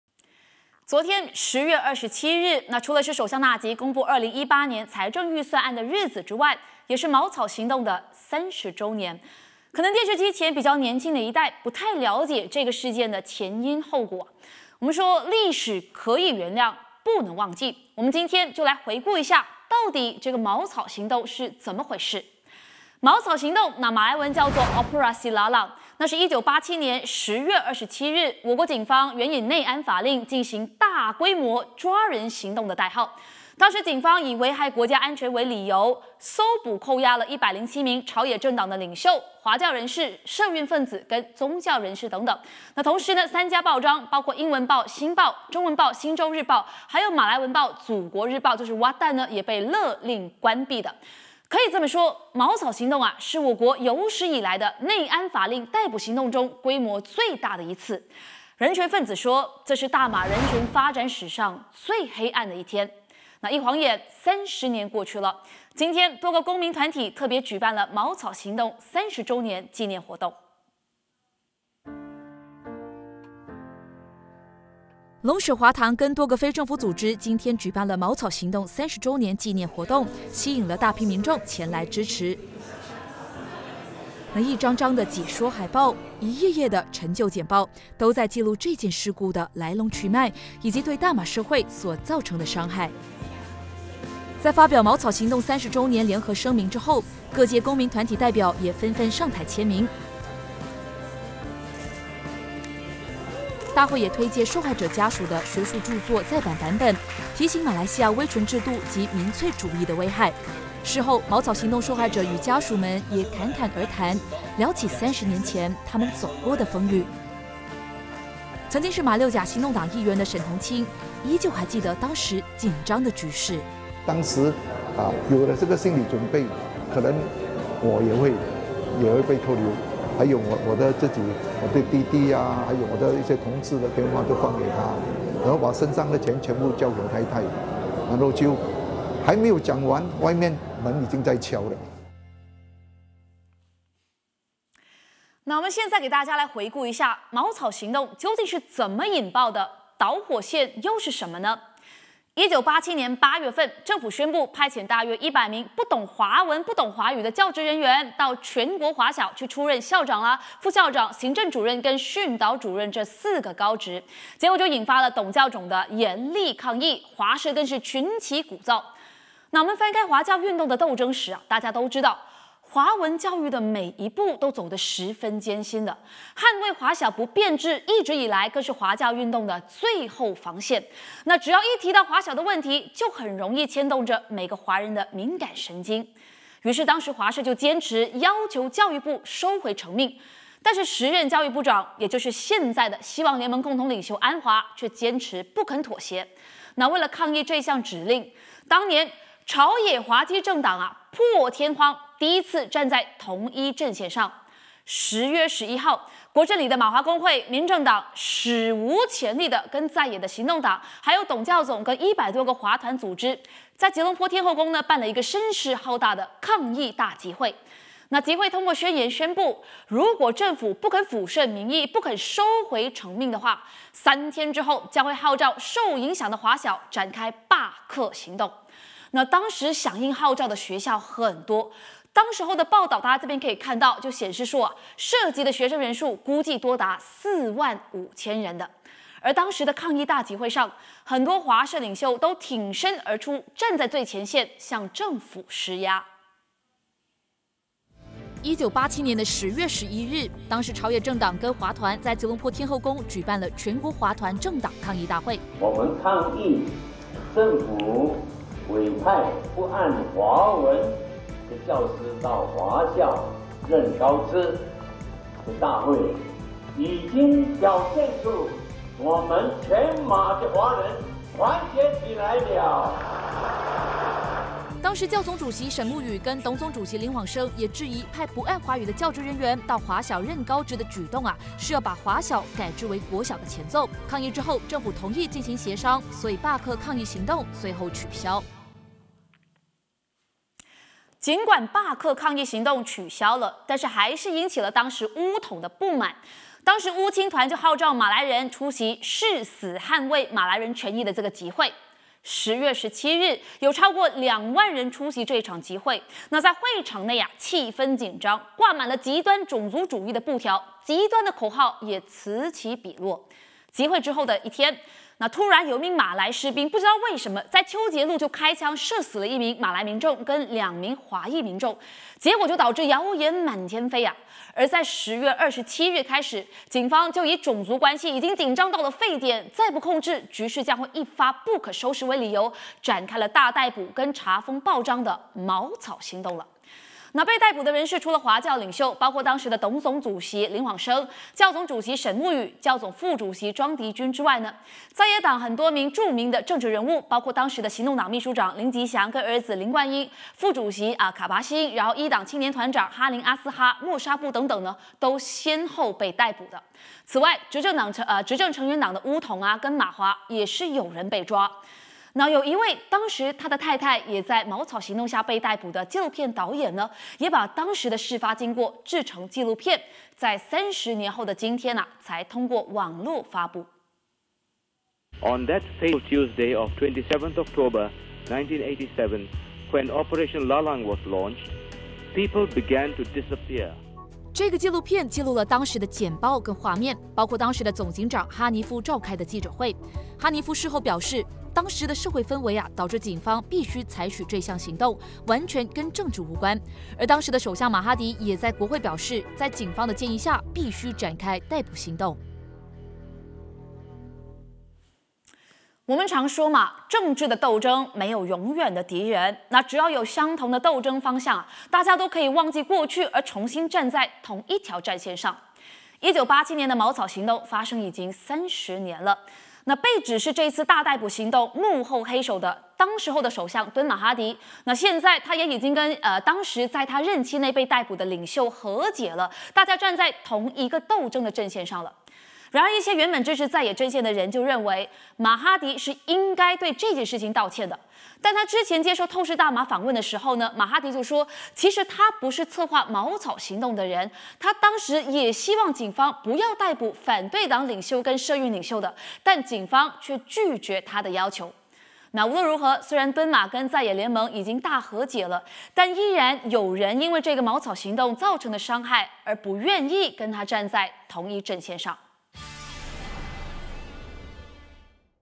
Prime Talk 八点最热报 28/10/17 - 回顾茅草行动30周年
1987年10月27日爆发的茅草行动Operasi Lalang，是我国政治与社会的分水岭，更是当代历史上最大规模的逮捕行动之一。30年过去，多个公民团体举办纪念活动，反思茅草行动为社会所带来的创伤。